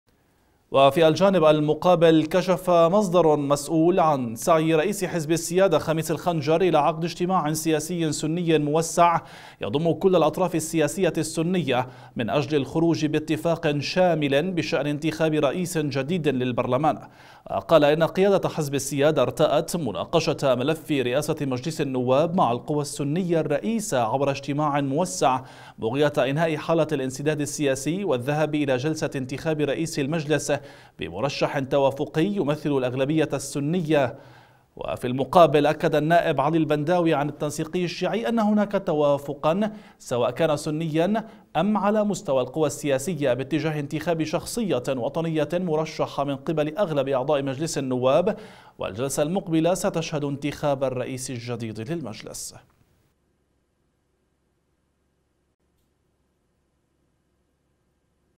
الخنجر يحاول جمع البيت السني على كلمة سواء
وفي الجانب المقابل كشف مصدر مسؤول عن سعي رئيس حزب السيادة خميس الخنجر الى عقد اجتماع سياسي سني موسع يضم كل الأطراف السياسية السنية من أجل الخروج باتفاق شامل بشأن انتخاب رئيس جديد للبرلمان، وقال ان قيادة حزب السيادة ارتأت مناقشة ملف رئاسة مجلس النواب مع القوى السنية الرئيسة عبر اجتماع موسع بغية إنهاء حالة الانسداد السياسي والذهاب إلى جلسة انتخاب رئيس المجلس بمرشح توافقي يمثل الأغلبية السنية، في المقابل أكد النائب علي البنداوي عن التنسيقي الشيعي أن هناك توافقا سواءً كان سنياً أم على مستوى القوى السياسية باتجاه انتخاب شخصية وطنية مرشحة من قبل أغلب أعضاء مجلس النواب والجلسة المقبلة ستشهد انتخاب الرئيس الجديد للمجلس.